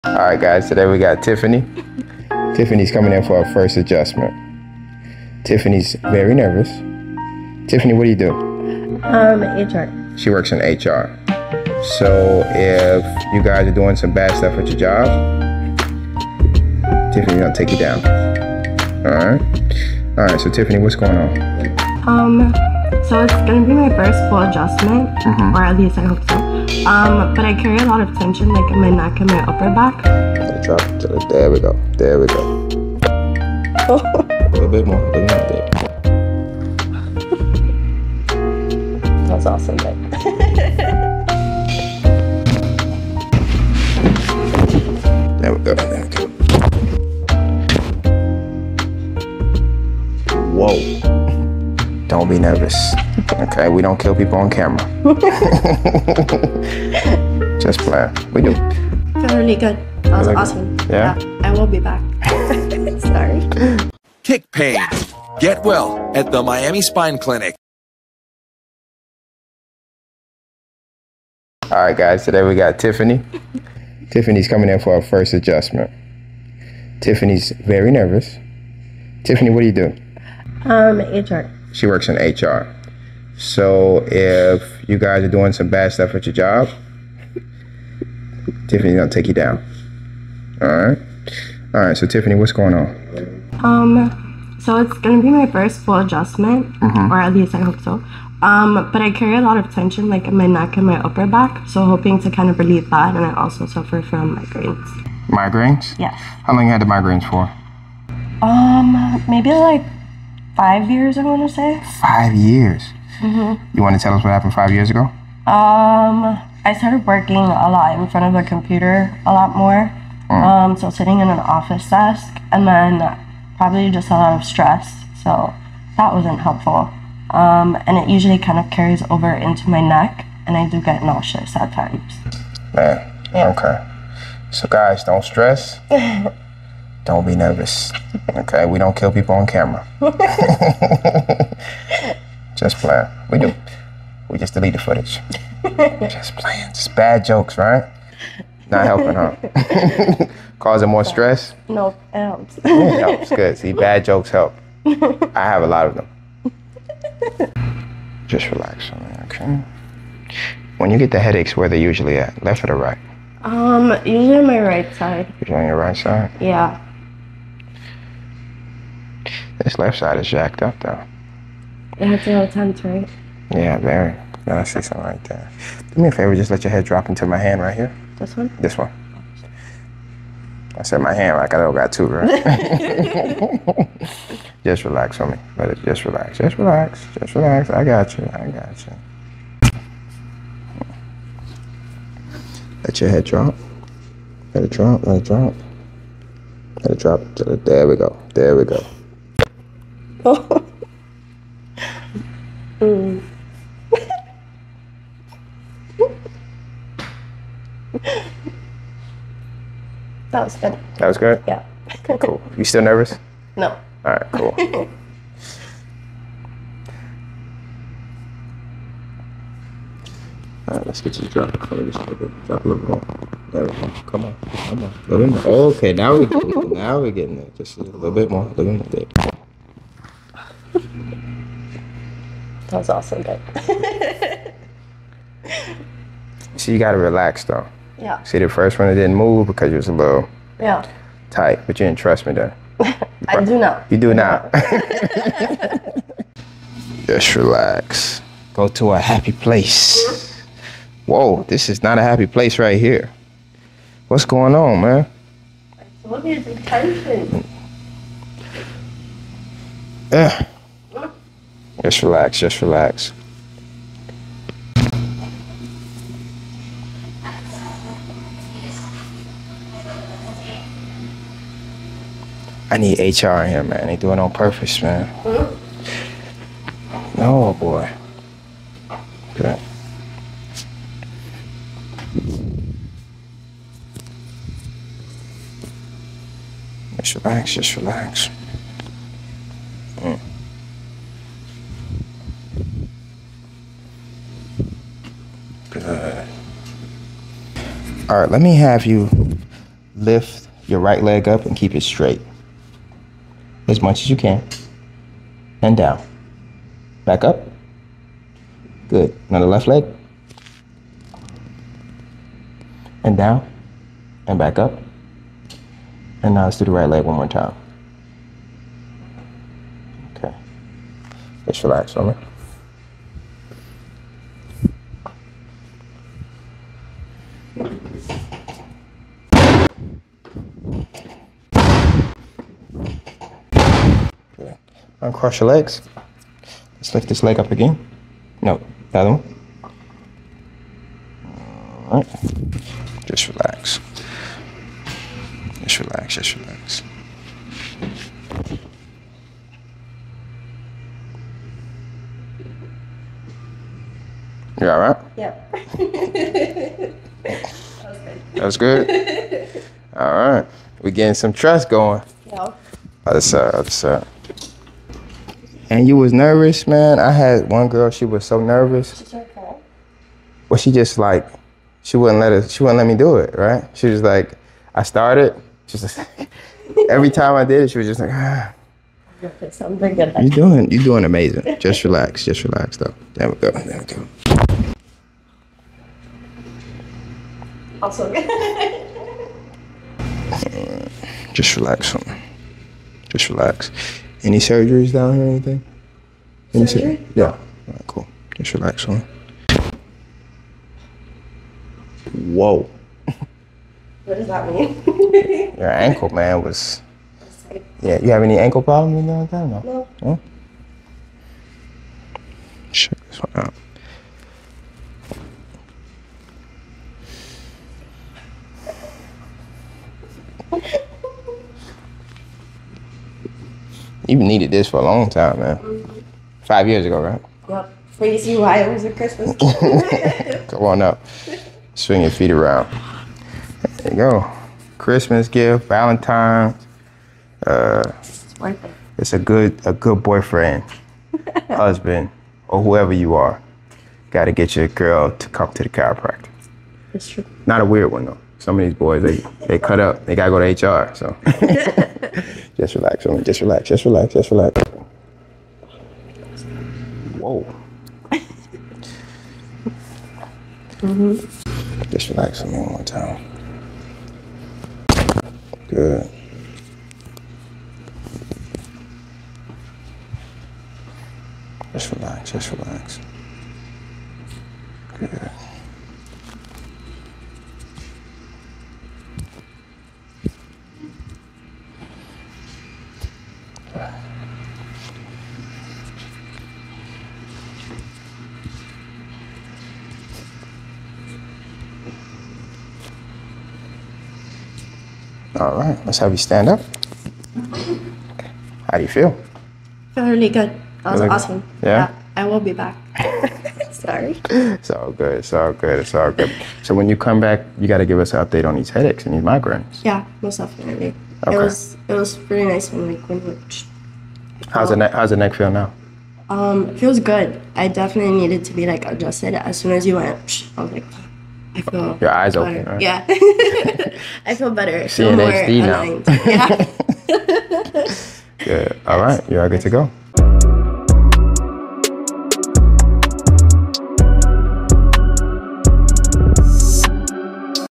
She was NERVOUS AND GRATEFUL AFTER THE ADJUSTMENT Chiropractor Adjust Nervous Patient
Tiffany came into our office with some neckpain and lowbackpain . She was very nervous, but that's perfectly normal. We had some great results with her treatment, and we hope to see her again very soon.
Facebook: Miami Spine Clinic/ Randall Lawrence Thompson
X/ Twitter: MiaSpineclinc